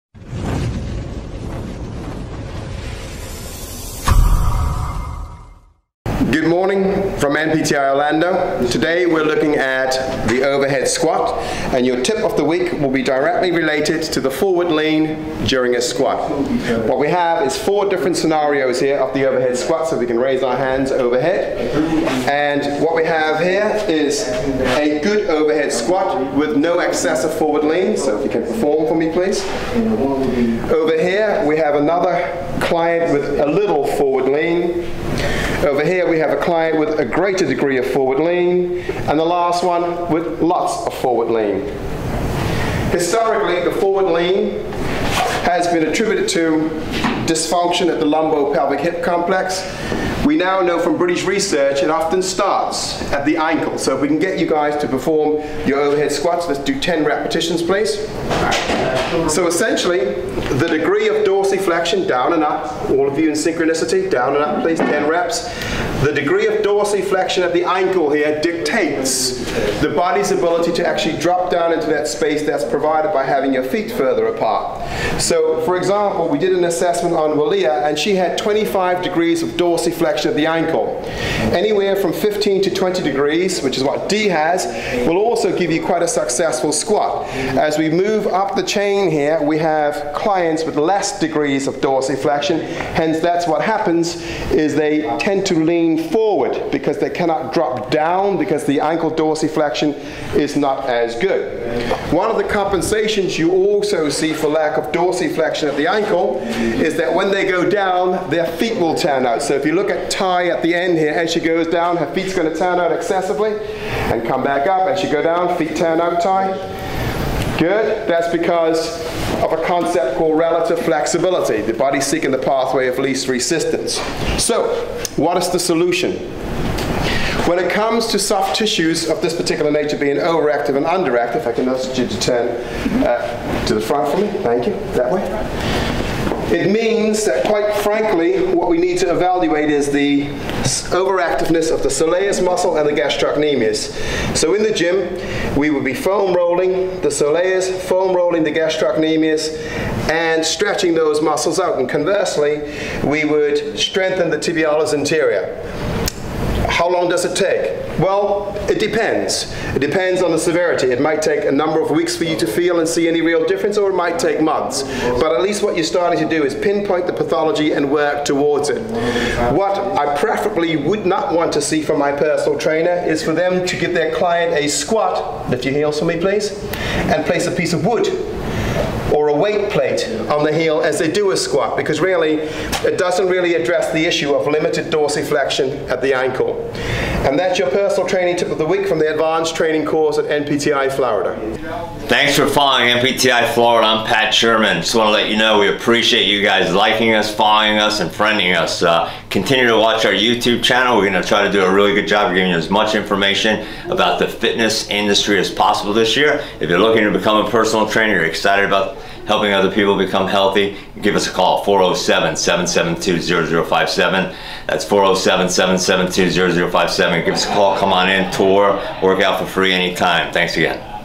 Health and Nutrition Coach program at NPTI Florida (Orlando)
NPTI Florida (Orlando) is the only 600 hour ACCSC Accredited Health and Nutrition Coach program in the US. This program is open to all industry personal trainers and NPTI Diploma graduates nationwide. Guaranteed to increase your personal training specialization and income.